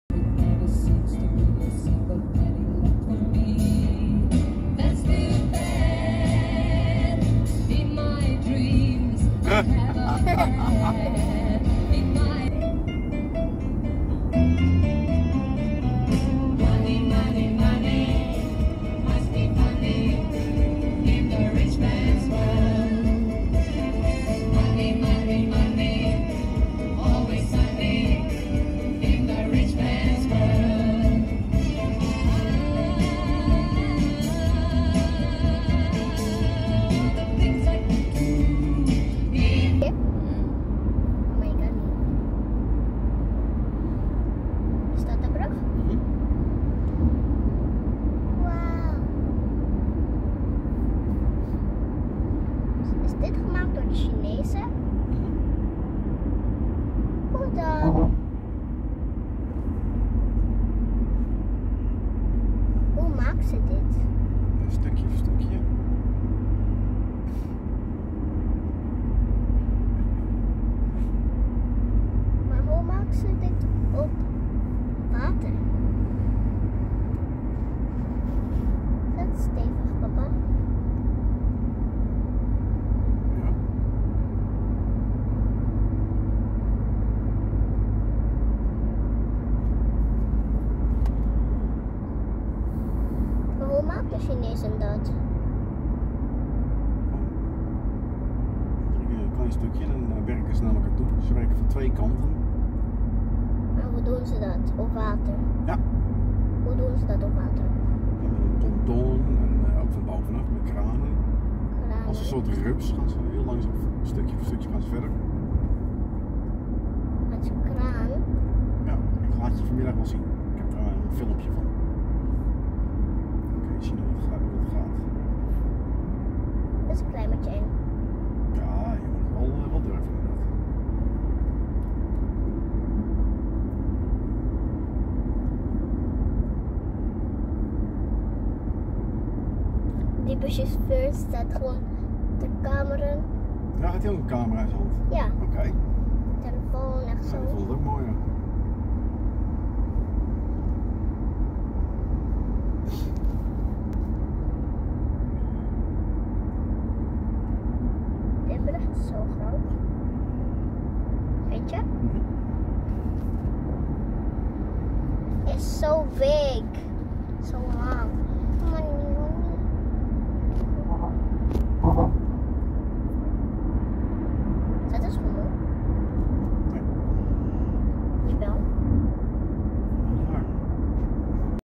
Crossing the newly opened Peljesac Bridge, Croatia|Travel & Wander with our Family|The Jemm's World
The Pelješac Bridge (Croatian: Pelješki most, pronounced [pěʎeʃkiː môːst]) is a cable-stayed bridge in Dubrovnik-Neretva County, Croatia. The bridge provides a fixed link from the southeastern Croatian semi-exclave to the rest of the country while bypassing Bosnia and Herzegovina's short coastal strip at Neum. The bridge spans the sea channel between Komarna on the northern mainland and the peninsula of Pelješac, thereby passing entirely through Croatian territory and avoiding any border crossings with Bosnia and Herzegovina at Neum.